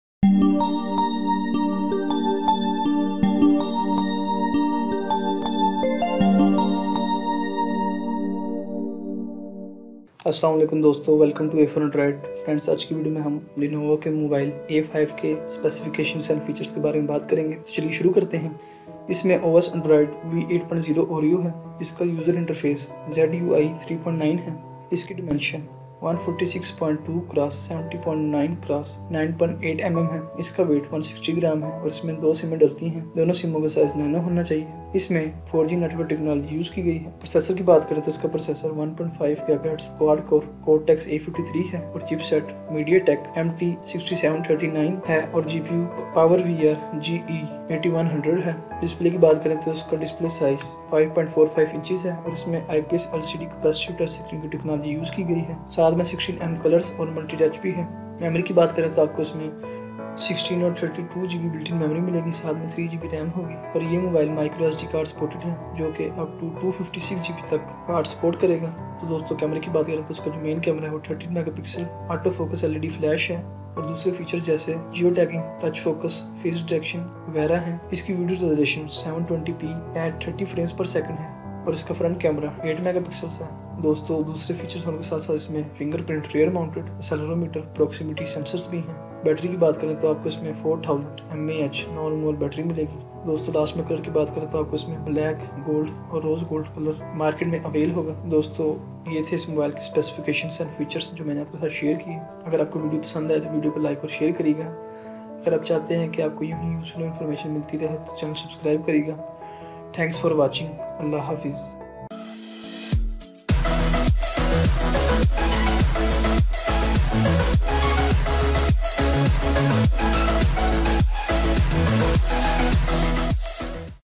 Lenovo A5 full Review (2018) | Lenovo A5 Full Specification and Features (Urdu/Hindi)
A.o.a .Welcome  to afor android...
In this video i discssed about the specification and features of Lenovo mobile A5..